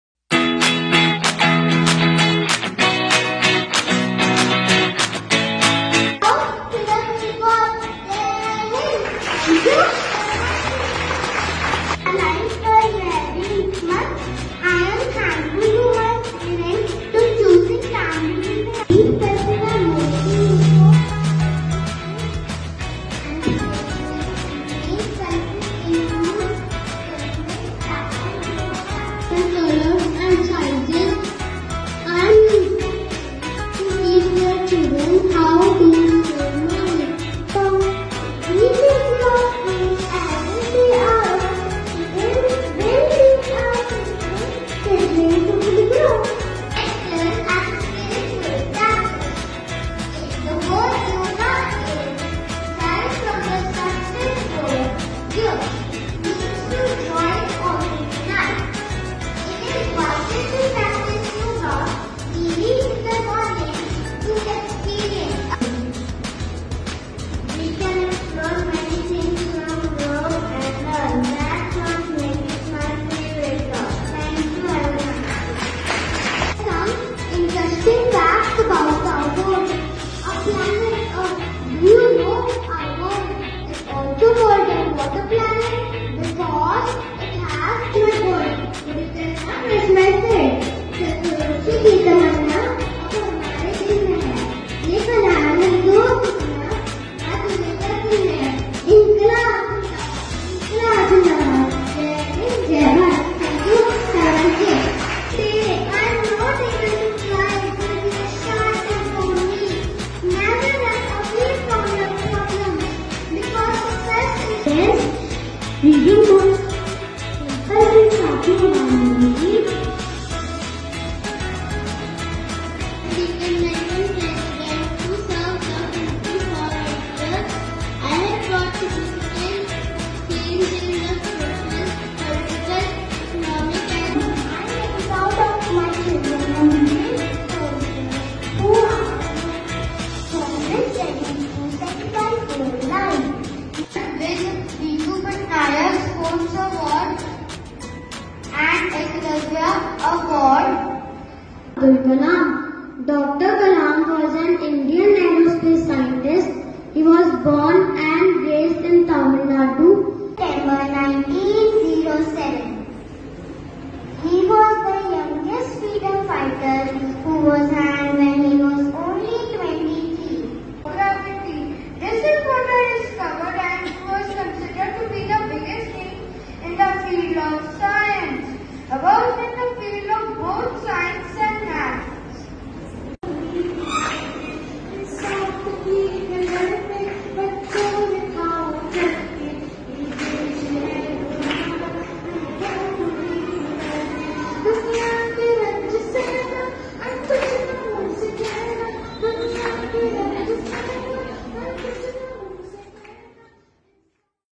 Express Yourself : Activity
We at Cambridge International School (CO-ED) believe in strengthening the verbal skills and fostering public speaking competency in young minds. Keeping this in mind “Express Yourself” activity was conducted for the students of Grade I-IV on July 29,2022 (Friday). The students showcased their oratory skills by putting forth their point of view with great zeal and enthusiasm. All the participants were awarded with certificates and prizes.
Acknowledging the efforts of the students, Our worthy President, Ms Pooja Bhatia applauded the students for their excellent efforts . The Vice Principal ,Ms Priyanka Grover appreciated the confidence of students at such a young age. Headmistress ,Ms Meenakshi Syal , Coordinators and Teachers
 wished them the best for their future endeavours.